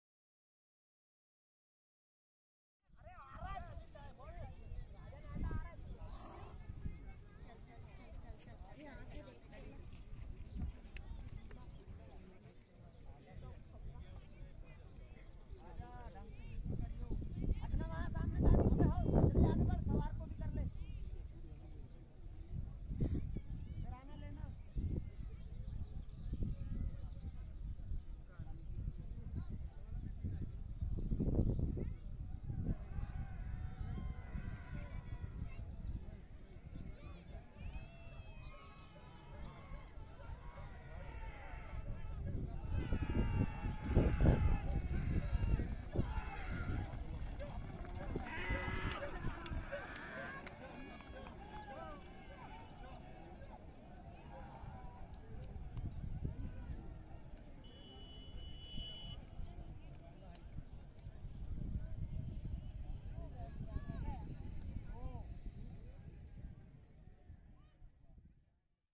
Mahesh set barghat vs ajim patel bihiriya kanhaiya ox race
Mahesh set barghat vs ajim patel bihiriya bailjodi race pat pratiyogita bhursitola barghat seoni madhya pradesh...

Channel creator - Raja tande